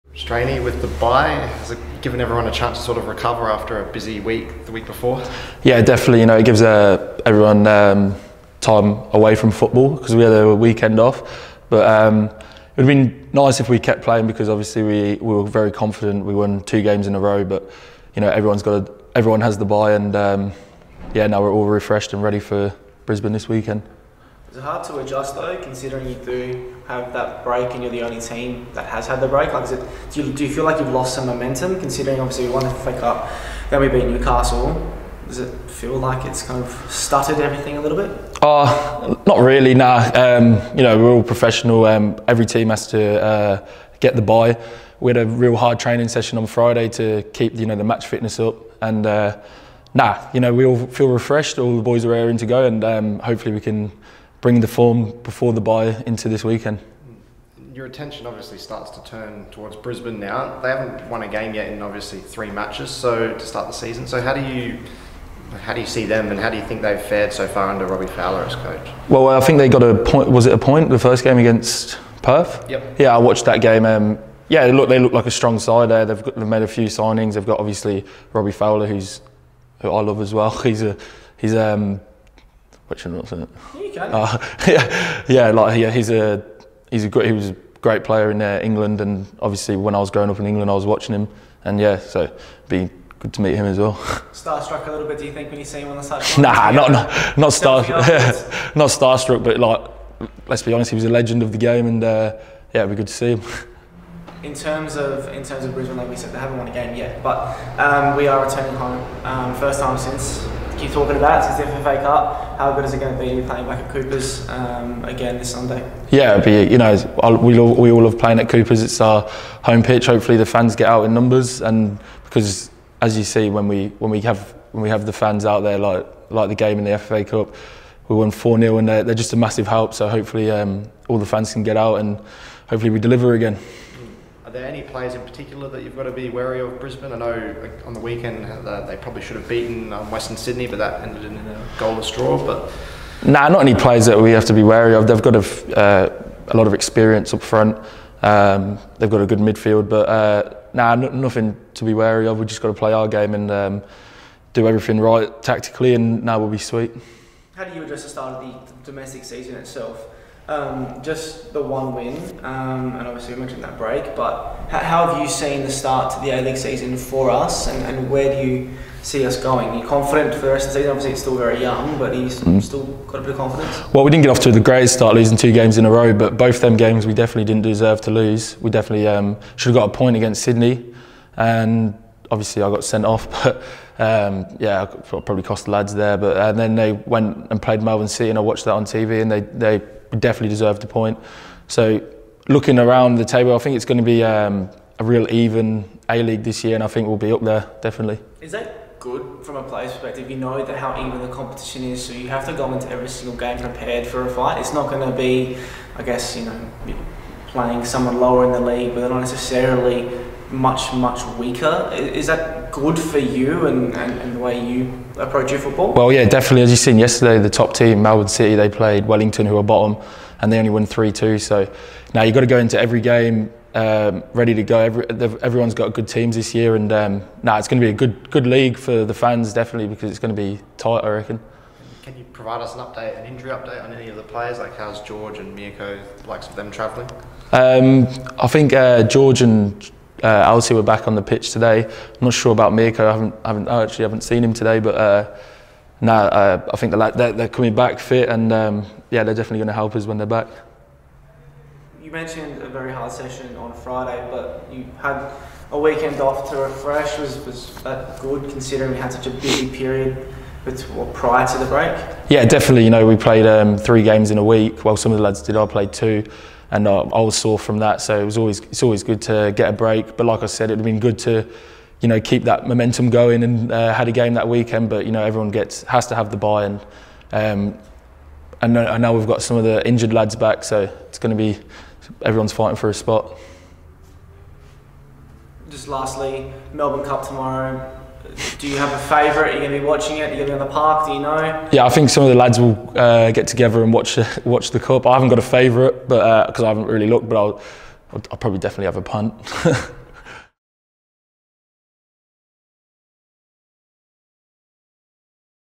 Ryan Strain Hyundai A-League 2019/20 Round 5 press conference
Ryan Strain speaks to the media at the Adelaide United Training Centre ahead of the Reds' Round 5 clash against Brisbane Roar at Coopers Stadium.